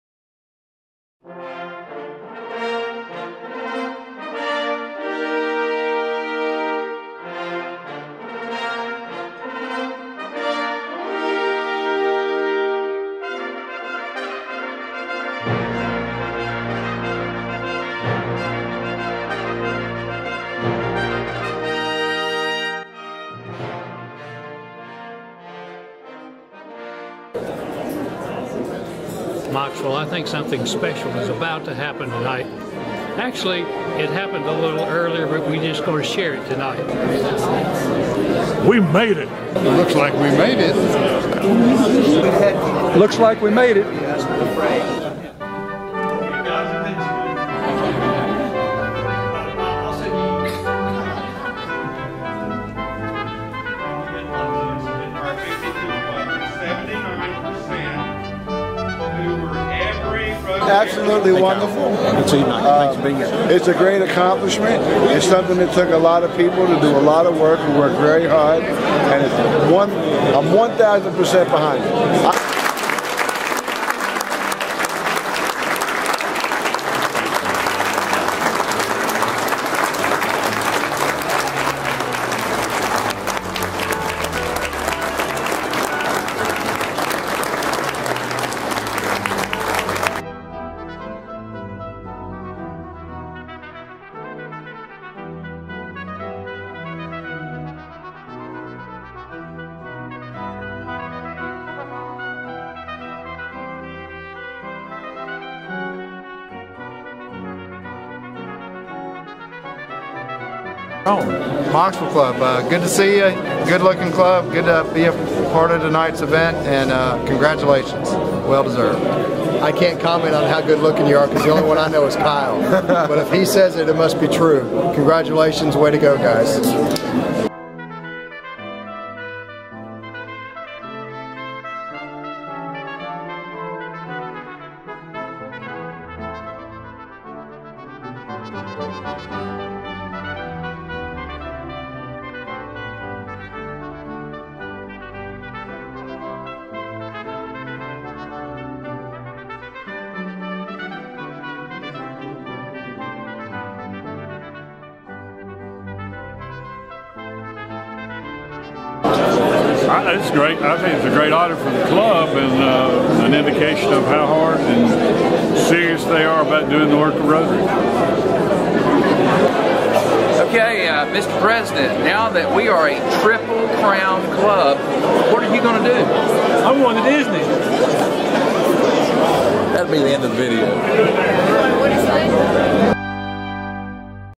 Mocksville Rotary Earns TRIPLE CROWN STATUS
Foundation Meeting in High Point, Mocksville honored as a Triple Crown Club